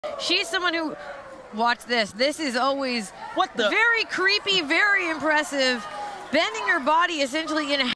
Vita VonStarr Uses Her Amazing Flexibility To Choke Dani Mo 😱
Vita Vonstarr Took On Dani Mo in a Match at MCW Spring Fever 2022 in Millersville, MD You can watch this show for FREE on MCW Rage TV, see links below

▶ New Full Matches Posted Every Monday Wednesday & Friday